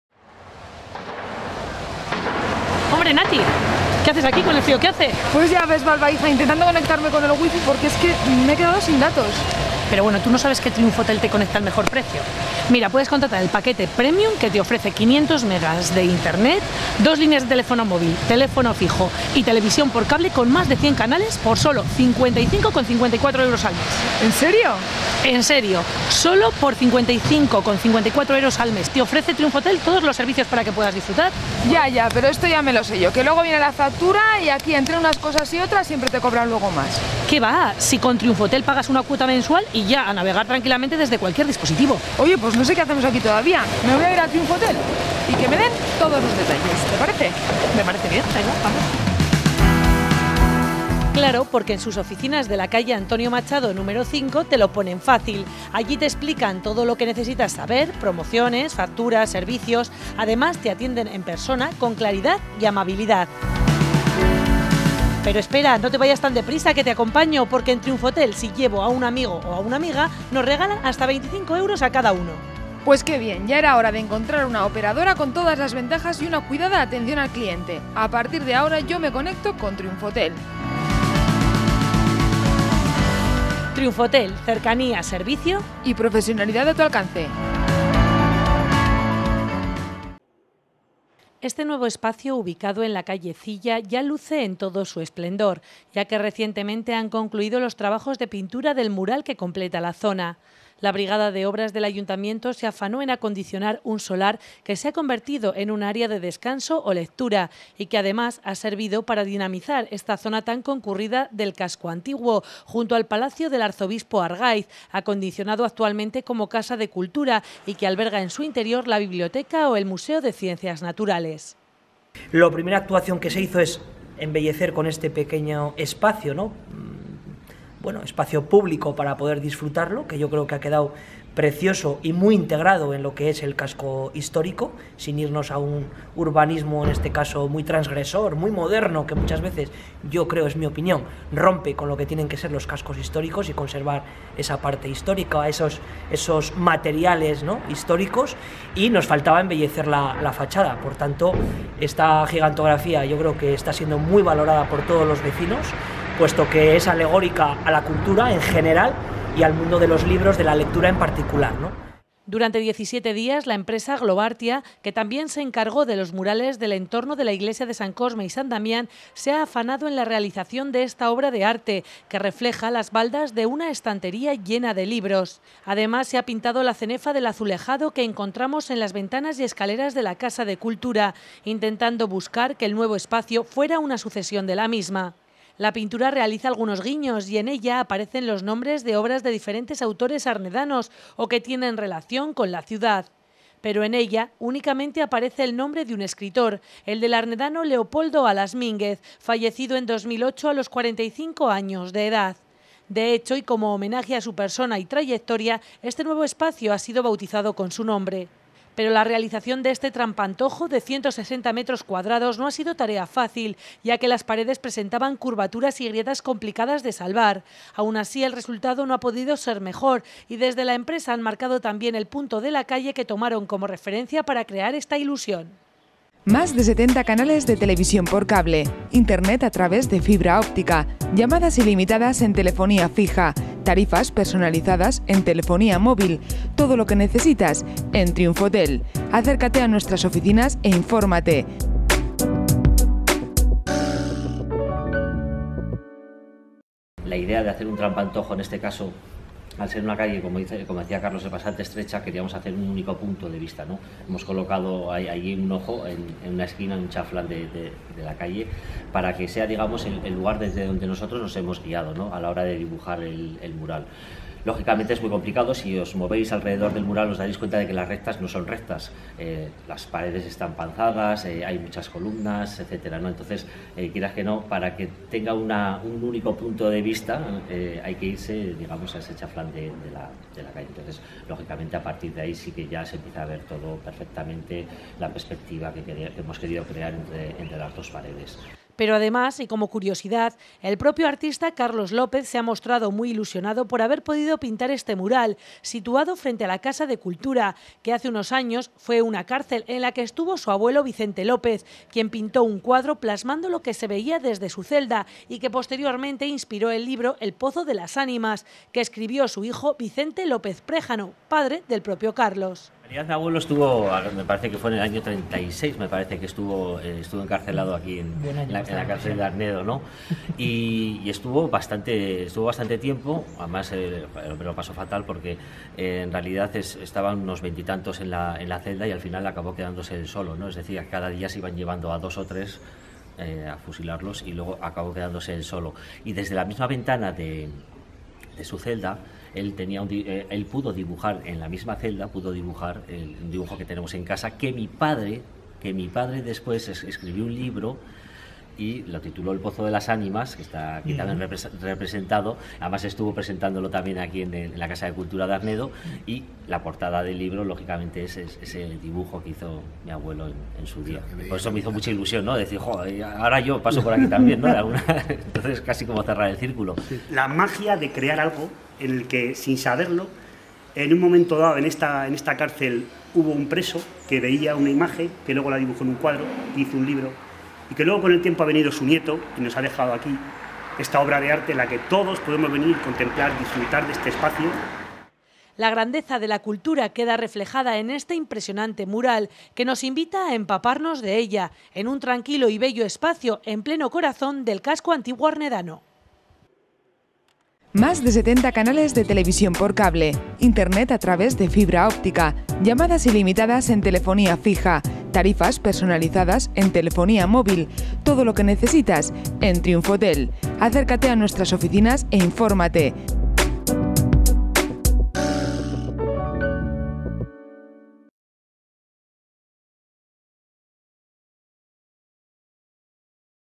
Nueva gigantografía junto a la Casa de Cultura
El Alcalde de Arnedo, Javier García, junto al concejal de Urbanismo y Obras, José Luis Rubio, presentaron el mural de gigantografía que recientemente se ha realizado en la calle Cilla, junto a la Casa de Cultura. Un nuevo espacio que recibirá el nombre del escritor Leopoldo Alas Mínguez, cuya biblioteca se encuentra precisamente en la propia Casa de Cultura, y que pretende convertirse en un área de descanso o lectura. Una actuación que además ha permitido recuperar y llenar de vida una nueva zona del Casco Antiguo.